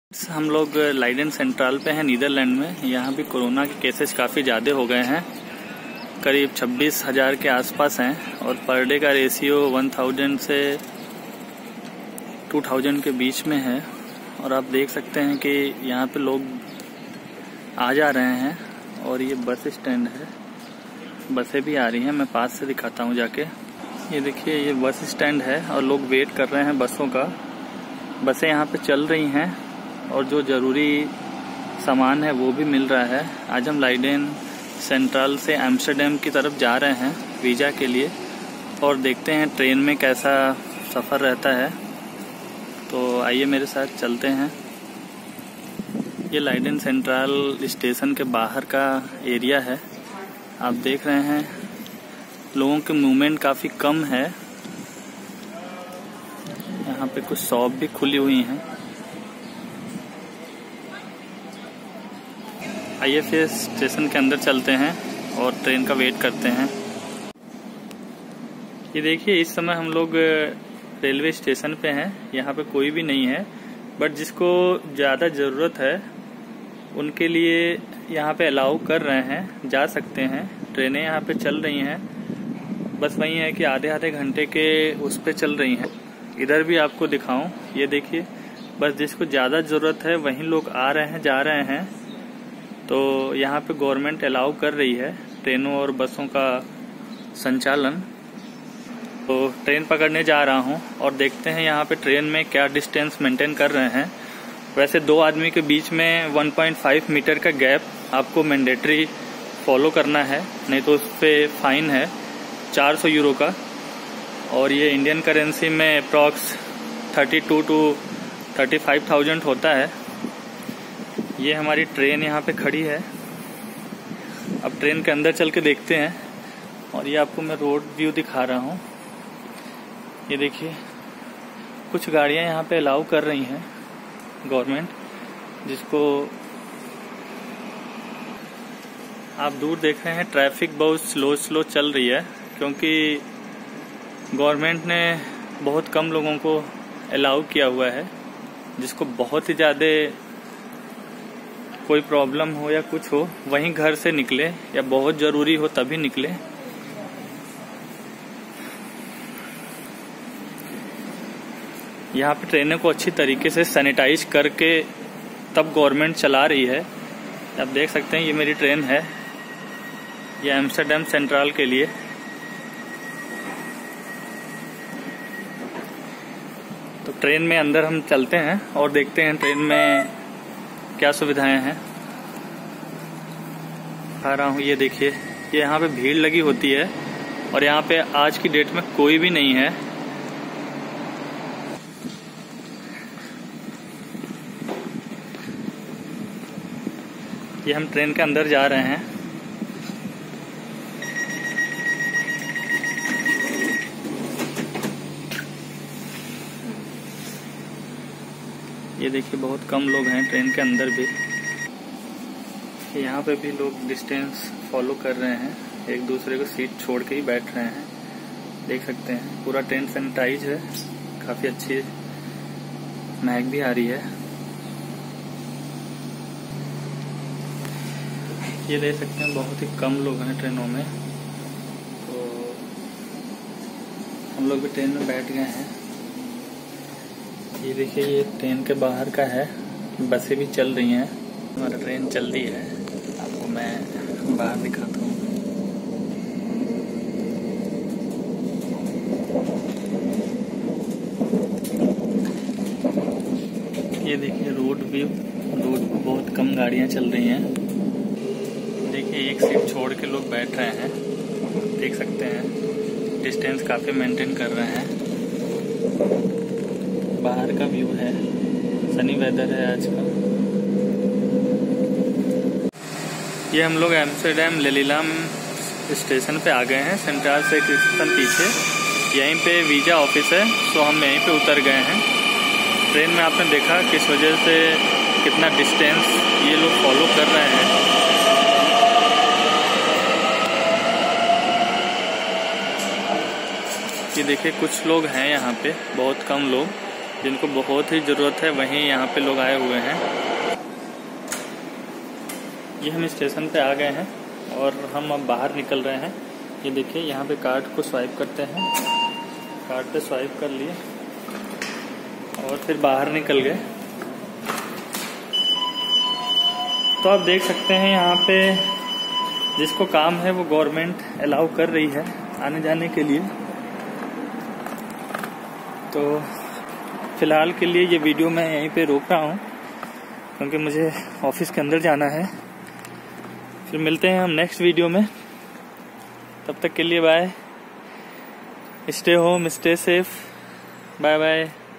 Train Journey to Amsterdam in LockDown Days लॉकडाउन का नीदरलैंड्स में क्या असर है देखिये
for more Fun & Press BELL Icon for More Upcoming Interesting Videos.